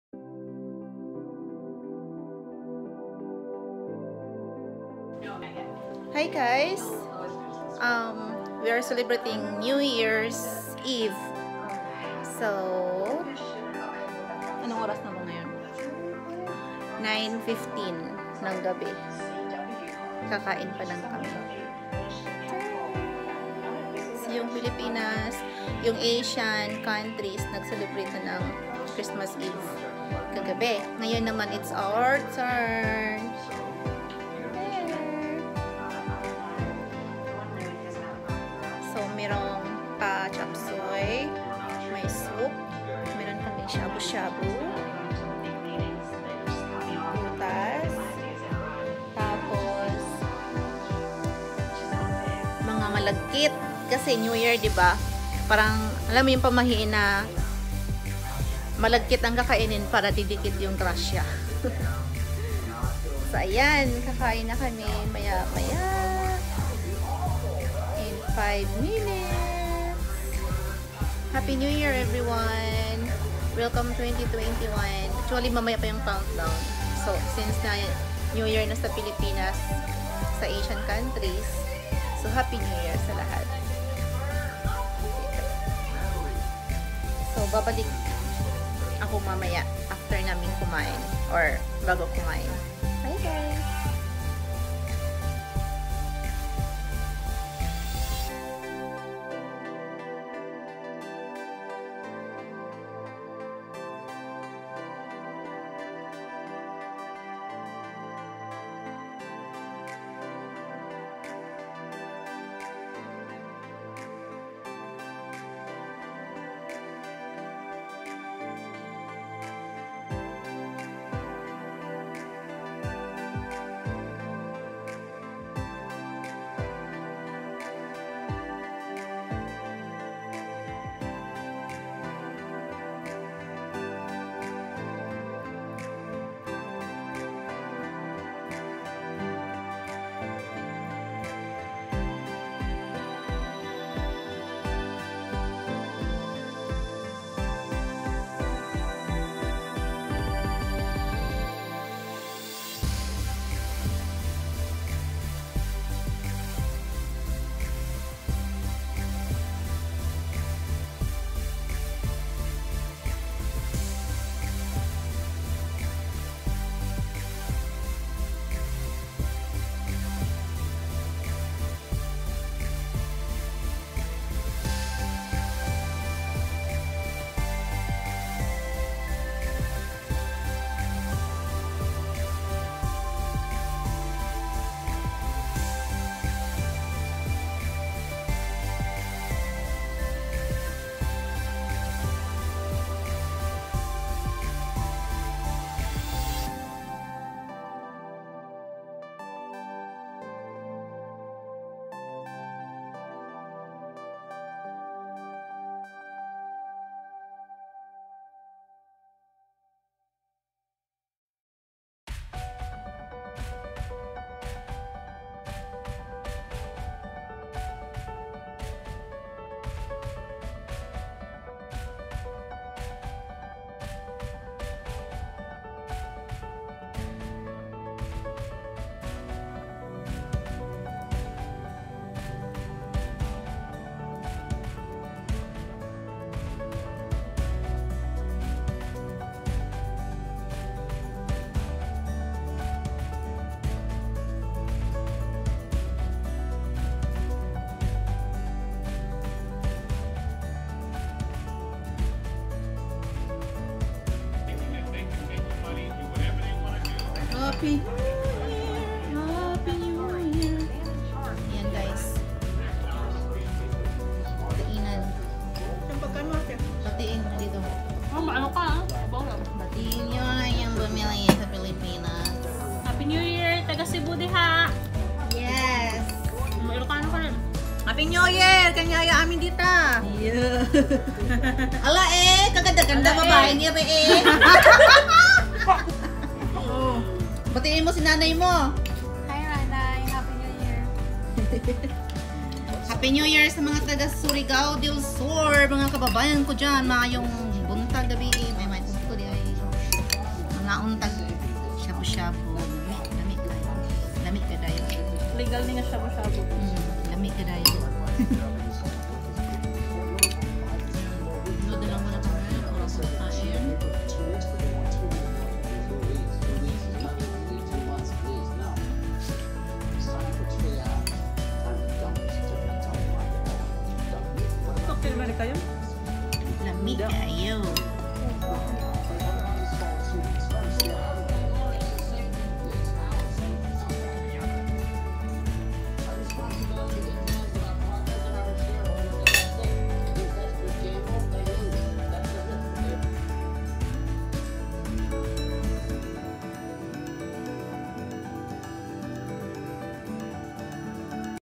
NEW YEAR'S EVE CELEBRATION | WELCOME 2021! | HOUSEMATES | LEE RHEY VLOGS
happy new year y'all! Late post super busy;)
God bless!